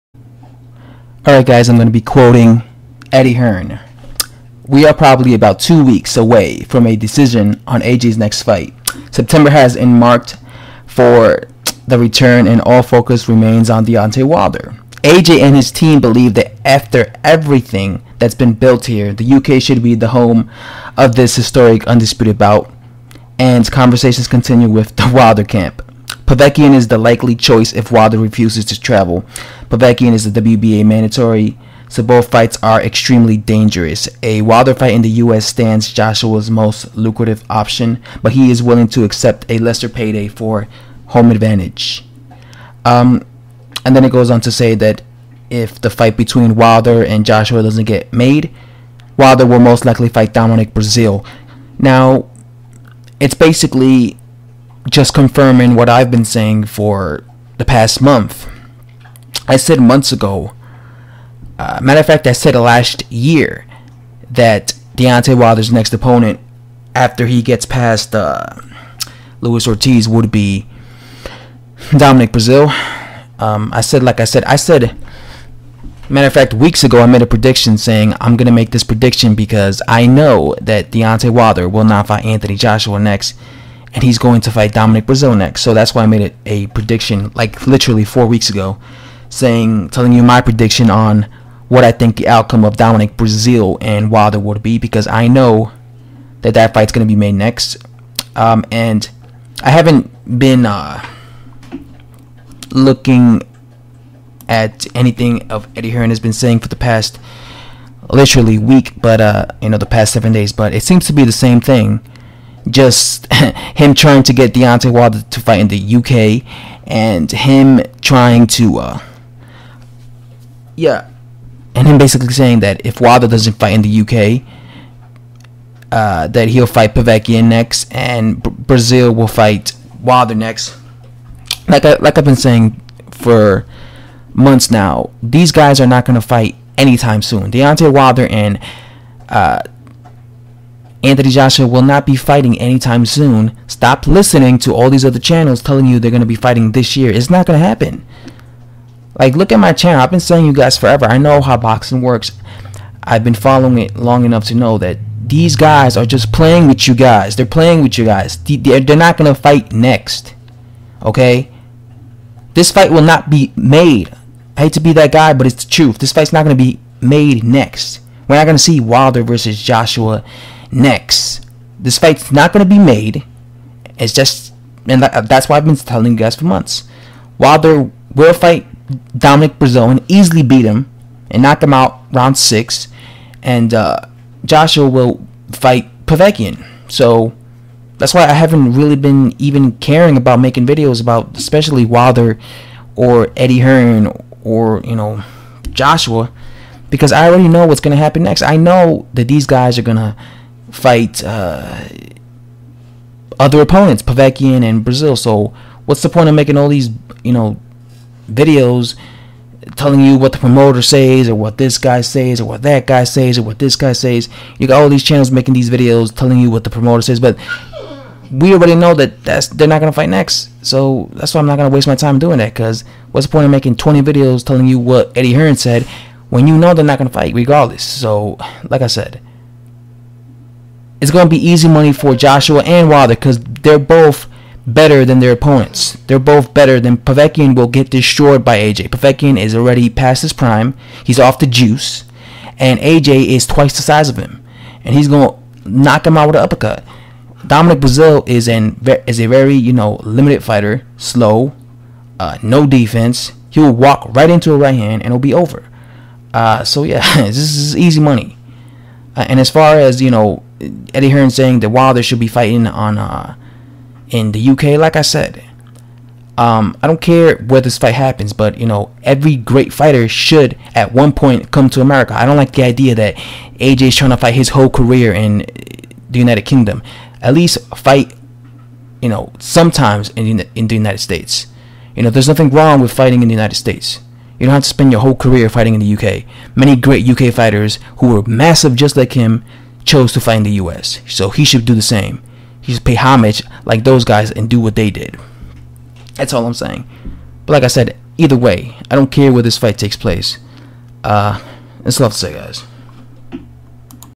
"IF WILDER DOES NOT TRAVEL TO THE UK FOR JOSHUA POVETKIN IS NEXT" EDDIE HEARN
Sub or Rot in The Depths Of Hell and burn to a crisp in Mount Dooom just kidding just kidding! lol